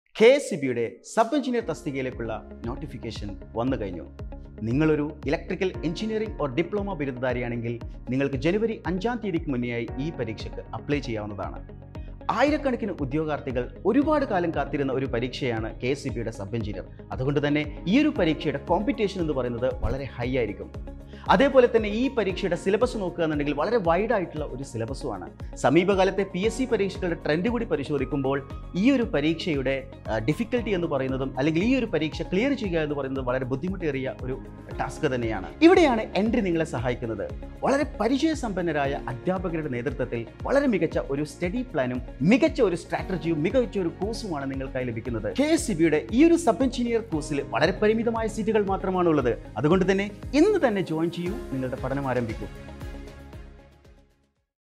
KSEB സബ് എൻജിനീയർ നോട്ടിഫിക്കേഷൻ വന്നു!
വളരെ കാലത്തിന് ശേഷം വന്നൊരു നോട്ടിഫിക്കേഷൻ ആണ്. തയ്യാറെടുപ്പും അത്ര ഗംഭീരമാക്കണം! കോമ്പറ്റിഷനും, സിലബസും, പ്രധാന കോൺസെപ്റ്റുകളും എല്ലാം മനസ്സിൽ വച്ചുകൊണ്ട് എൻട്രി ആപ്പ് ഉദ്യോഗാർഥികൾക്കായി ഒരു സ്പെഷ്യൽ KSEB സബ് എൻജിനീയർ കോഴ്സ് തുടങ്ങുന്നു. വളരെ പരിമിതമായ സീറ്റുകൾ മാത്രമേ ഈ കോഴ്സിൽ ഉള്ളു. ഉടൻ ജോയിൻ ചെയ്യൂ!